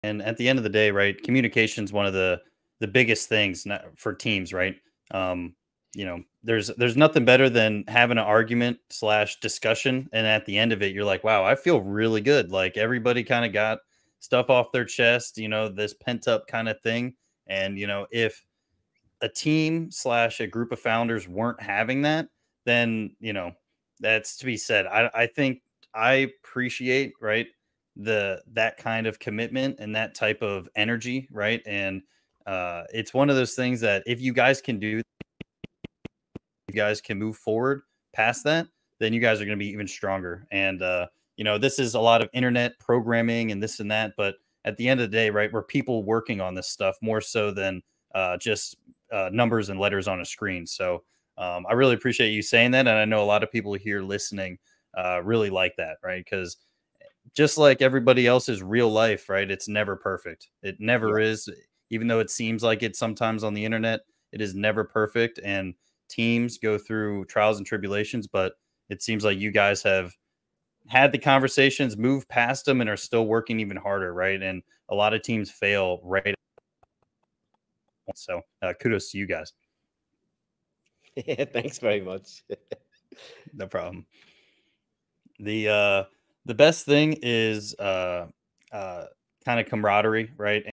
Building Stronger Teams: The Power of Communication and Commitment
Discover the importance of open communication and commitment within teams. Learn how having constructive arguments and discussions can strengthen your team and foster a sense of camaraderie. Don't miss out on valuable insights for building a successful team.